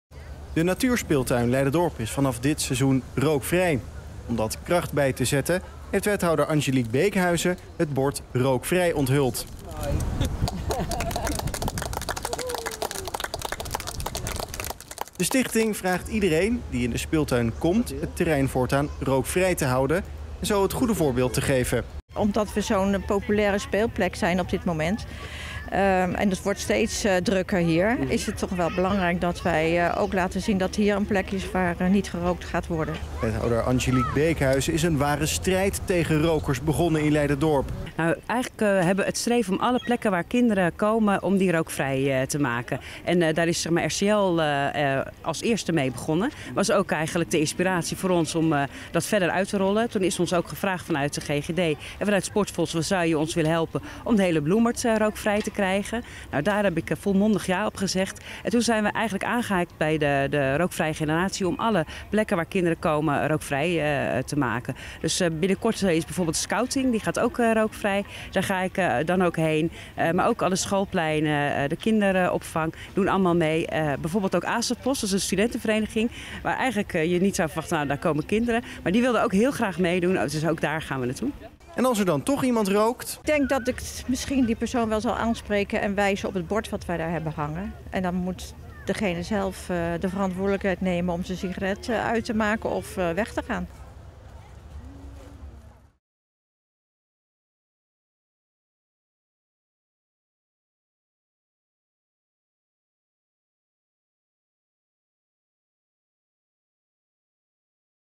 Natuurspeeltuin Leiderdorp rookvrij
LEIDERDORP - Donderdag is in de Natuurspeeltuin in Leiderdorp rookvrij verklaard. Wethouder Angelique Beekhuizen was aanwezig om het bord rookvrij te onthullen.

Voorzitter Riny Vons is erg blij dat ook de Natuurspeeltuin rookvrij is. 'Omdat we een populaire speeltuin zijn, is het toch ook belangrijk dat we laten zien dat hier niet gerookt mag worden'

Wethouder Beekhuizen is op dit moment bezig om meerdere plekken rookvrij te maken. 'We zijn bezig met nog een school, een scoutingvereniging en een studentenvereniging', aldus Beekhuizen. Kortom: een rookvrij Leiderdorp voor de kinderen.